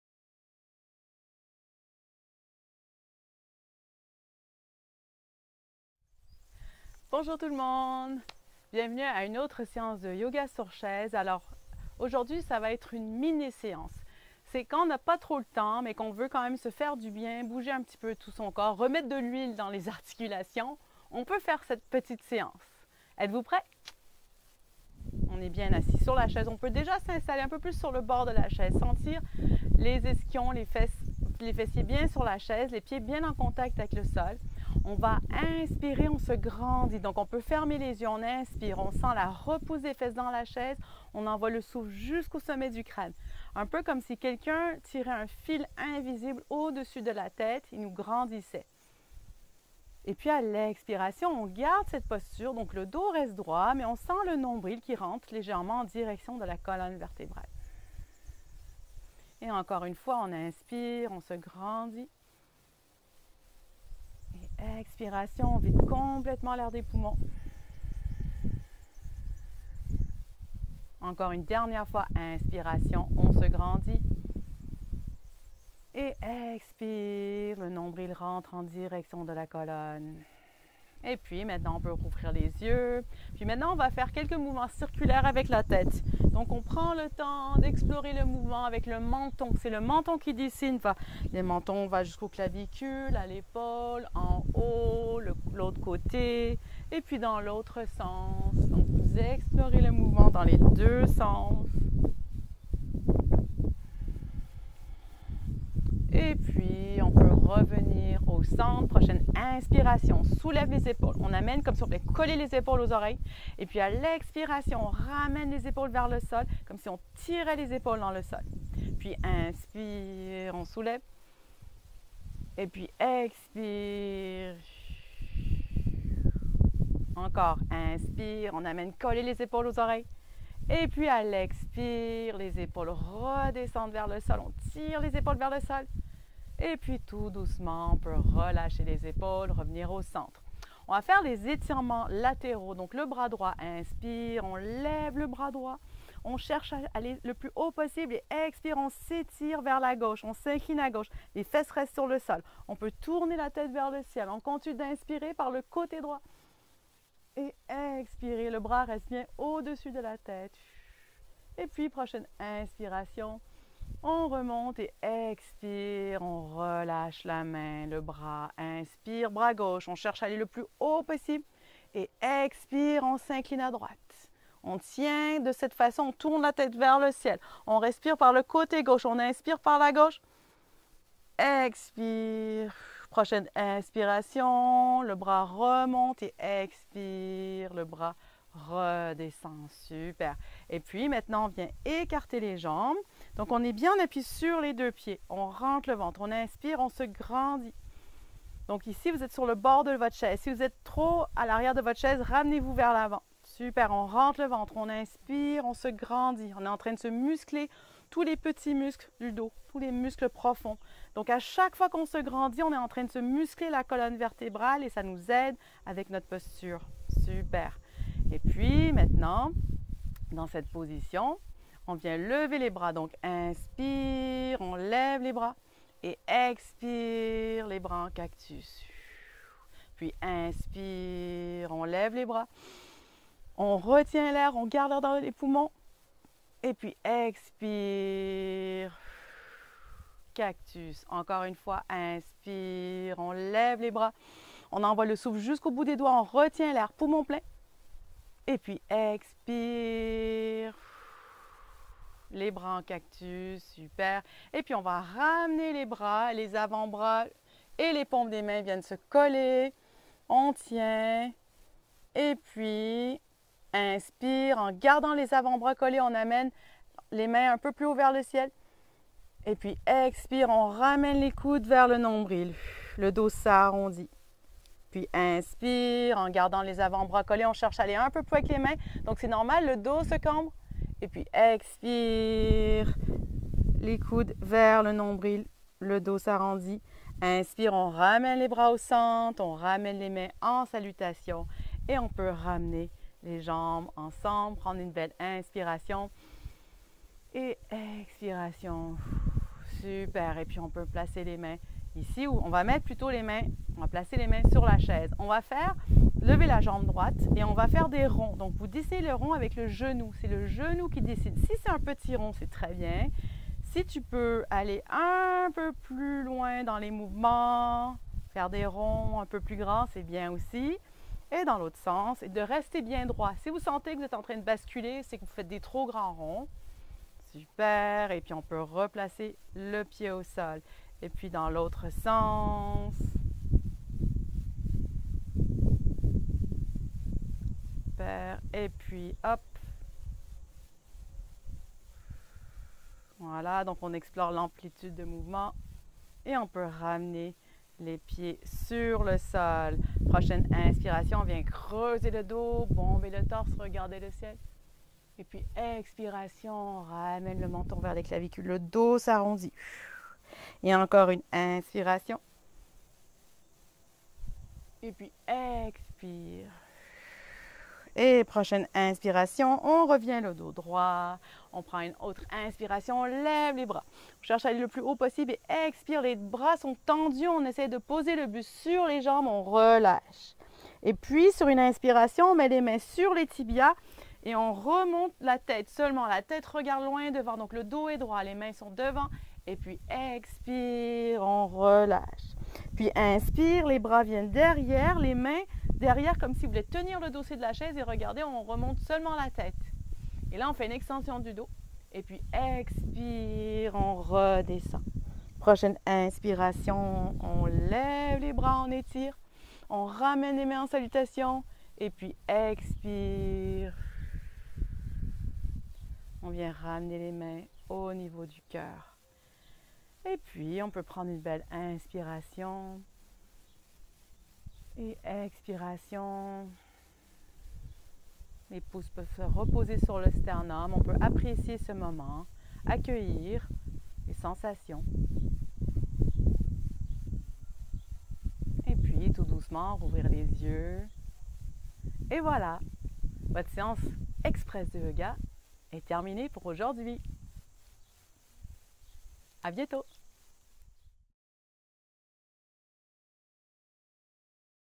Yoga sur chaise séance express
Voici une petite séance de yoga sur chaise que vous pouvez faire à tout moment de la journée.